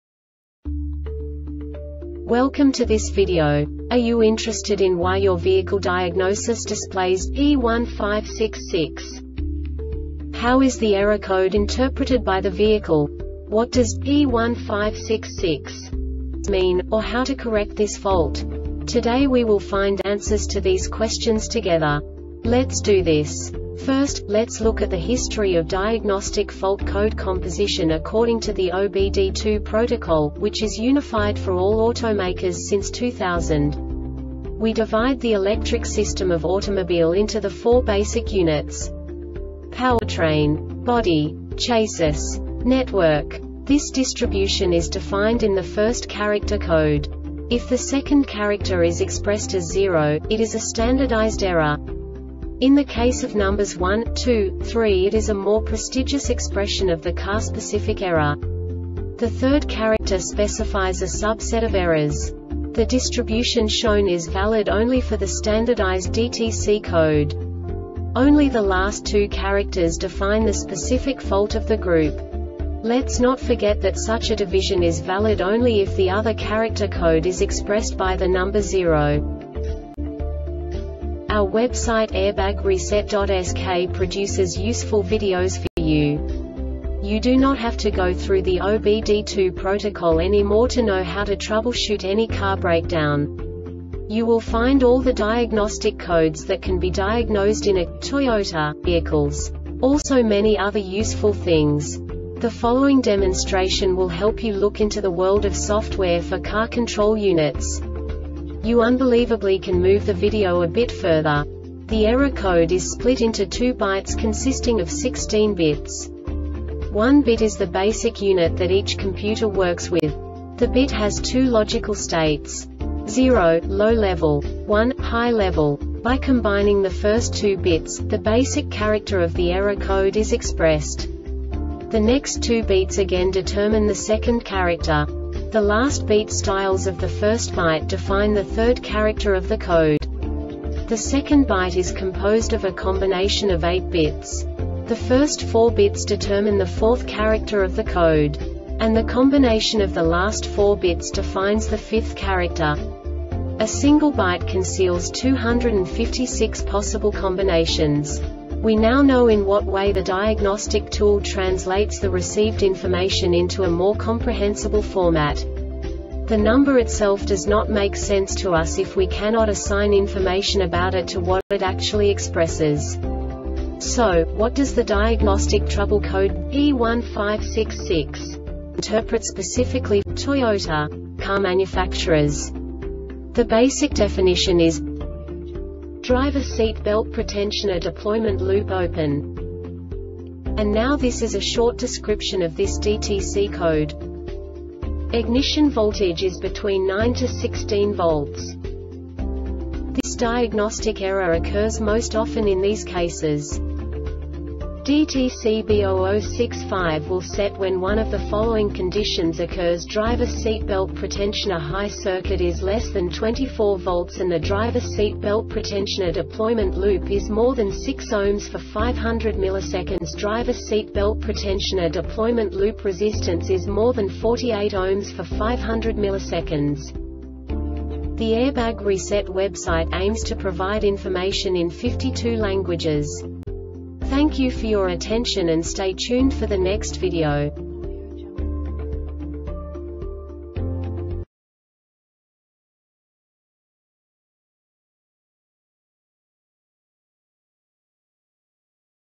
DTC Toyota P1566 Short Explanation
Contents:
0:21 Basic DTC analysis according to OBD2 protocol standard.
1:48 Insight into programming
2:58 A diagnostically understandable description of the Diagnostic Trouble Code
3:05 Basic error definition

Make:
Toyota
Code:
P1566
Definition:
Driver Seat Belt Pretensioner Deployment Loop Open
Description:
Ignition voltage is between 9-16 V.
Cause:
DTC B0065 will set when one of the following conditions occurs:Driver seat belt pretensioner high circuit is less than 2.4 V and the driver seat belt pretensioner deployment loop is more than 6 Ω for 500 milliseconds.Driver seat belt pretensioner deployment loop resistance is more than 4.8 Ω for 500 milliseconds.